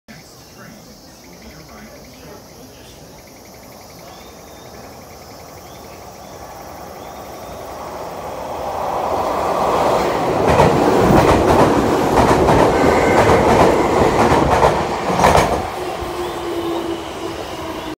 京王1000系1705編成『バイオレット』が急行として駒場東大前を通過するシーン
撮影場所は駒場東大前駅